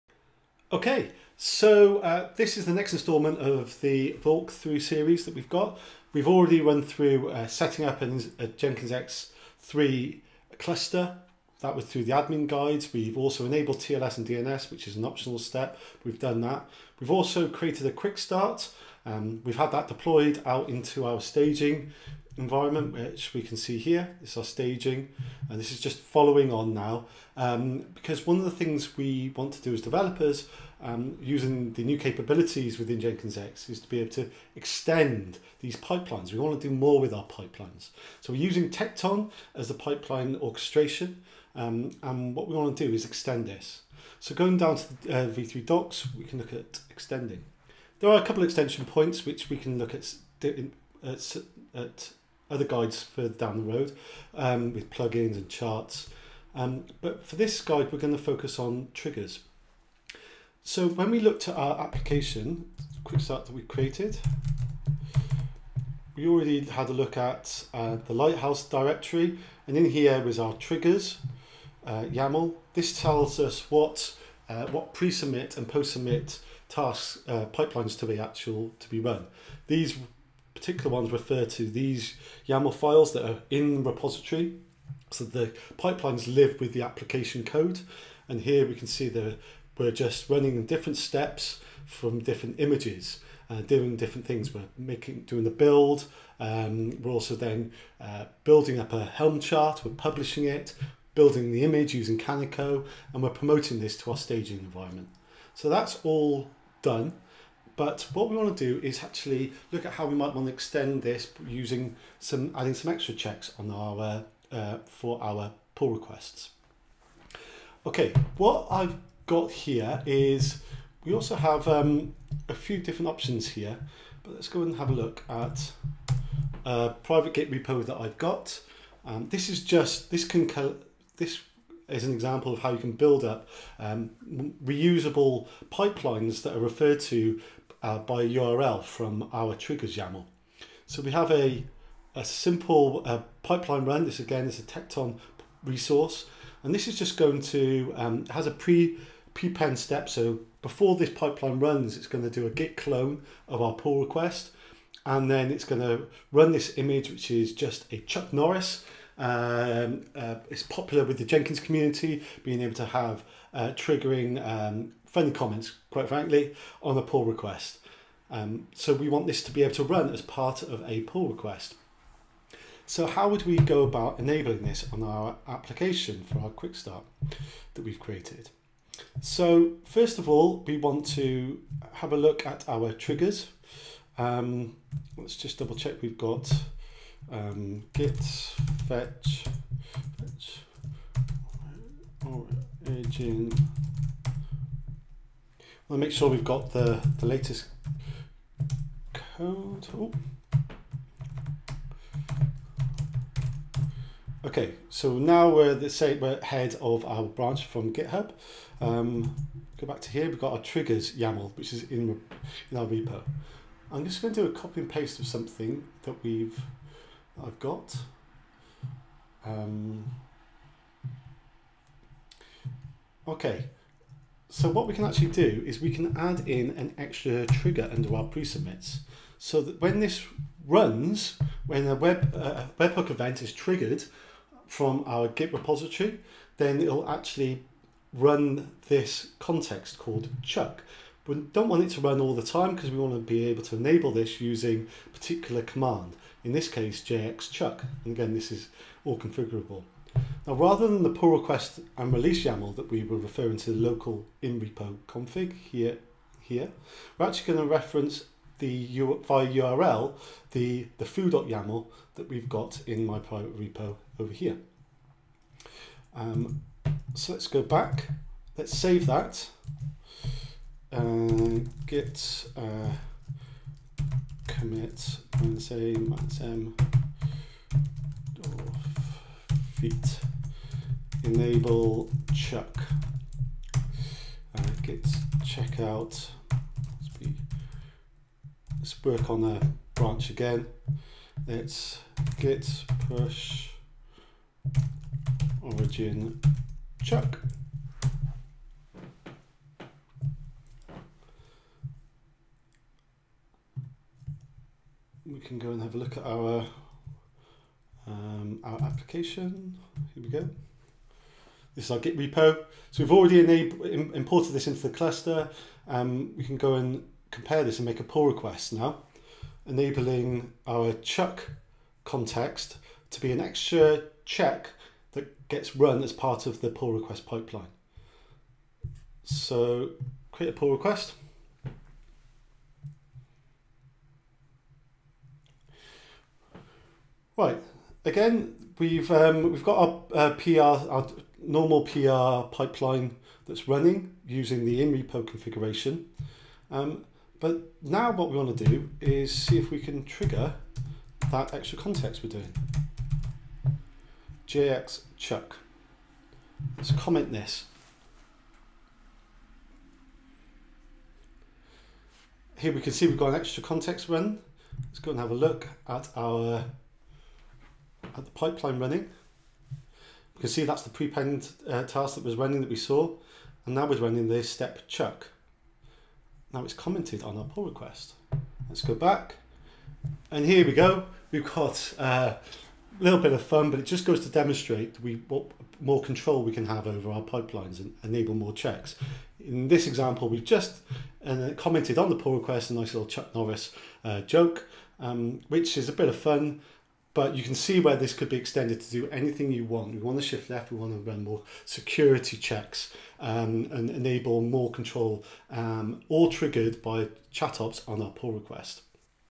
Jenkins X 3 Walkthrough - Trigger Tekton pipelines with Chuck Norris example
This guide shows how to add custom Tekton Pipelines to your applications using Jenkins X.  This demo will show the popular Chuck Norris comments being added to a Pull Request using Tekton pipeline resources.